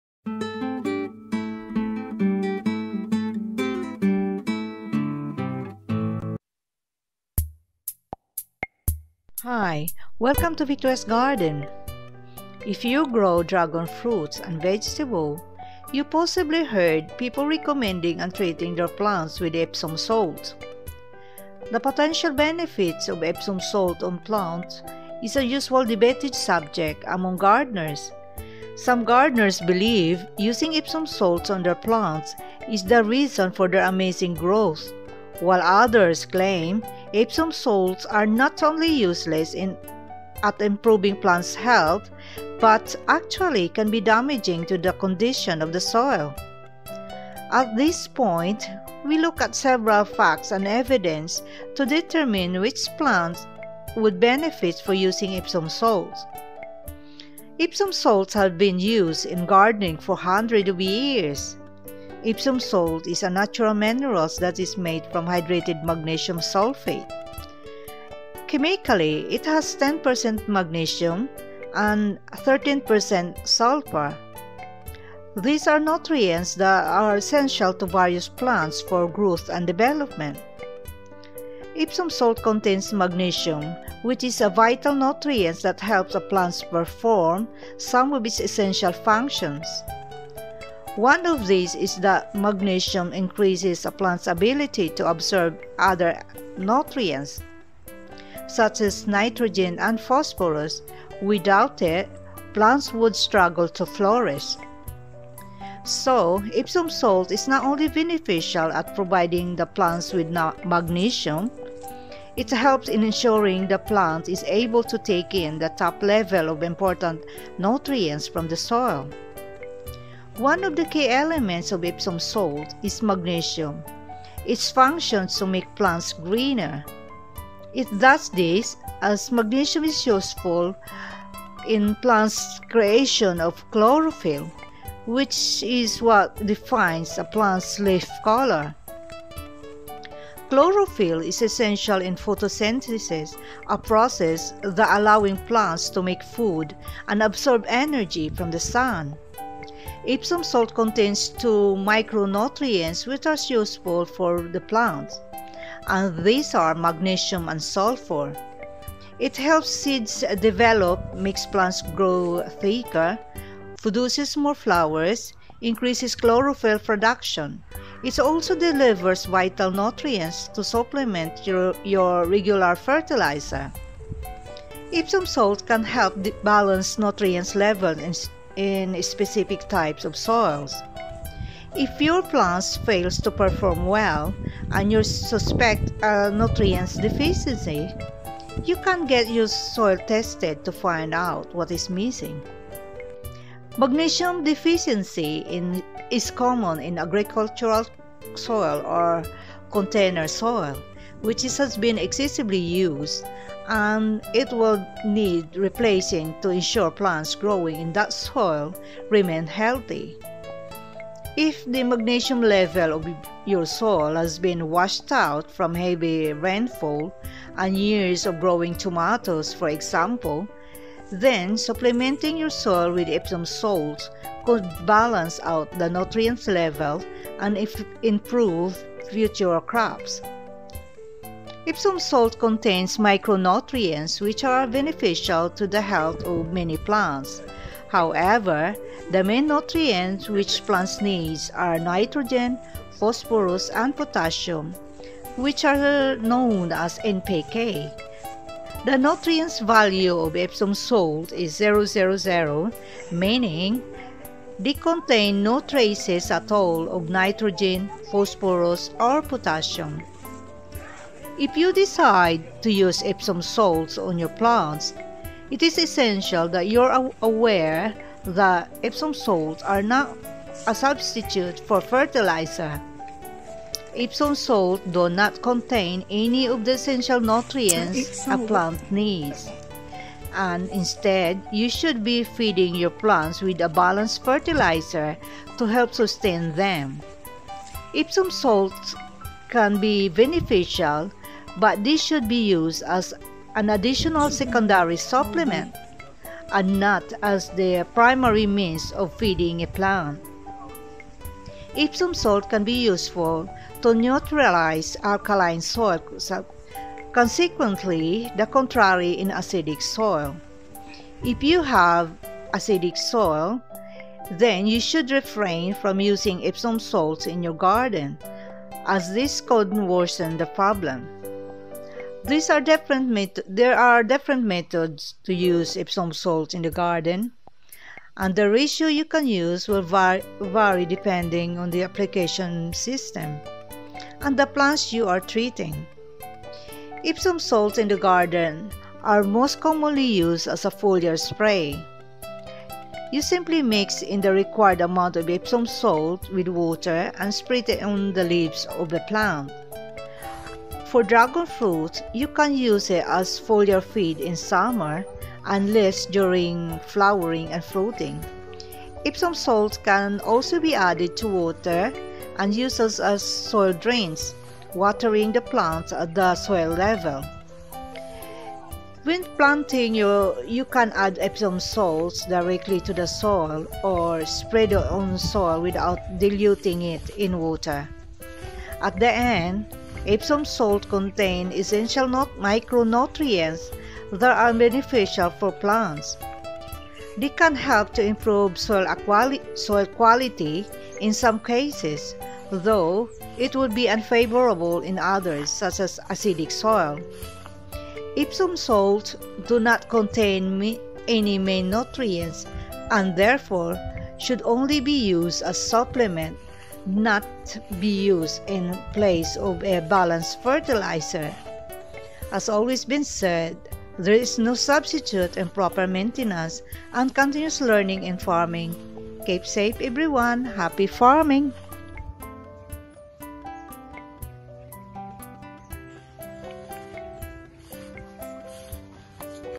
VG40: USES OF EPSOM SALTS ON DRAGON FRUIT AND OTHER PLANTS
I'm shaing the Uses of Epsom salts to Dragon fruits ,  plants and vegetables.